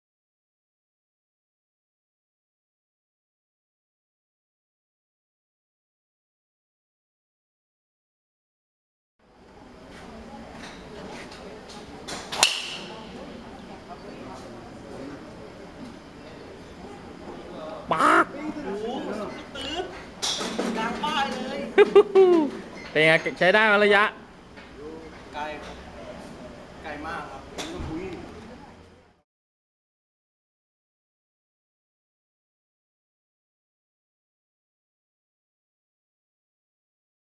Kami Driver Test Sang
ทดสอบหัวไม้หน้าเด้ง Kami โดยนายแซง ชนป้ายดังลั่น จากที่ใช้ไม้ตัวเองไม่ชน แสดงถึงความไกลจริง